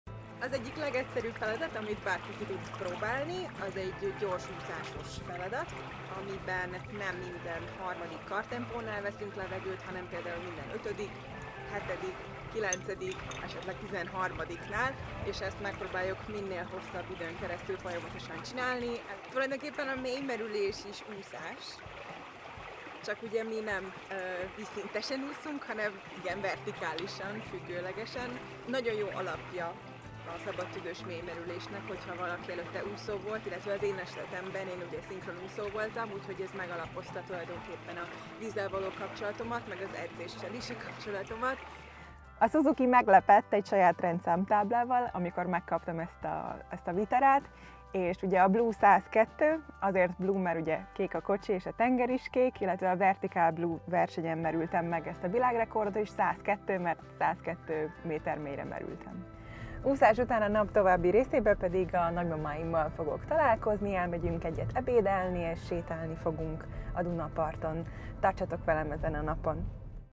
Egy nap Korok Fatimával és a Vitara limitált kiadásával - előzetes
Korok Fatima, a szabadtüdős mélymerülés női világrekordere és négyszeres világbajnoka lett limitált Vitaránk reklámarca! Óriásplakátok százai, TV reklámfilm és a java még csak most kezdődik! Kövesd végig Fatima egy napját és annak legjobb pillanatait te is!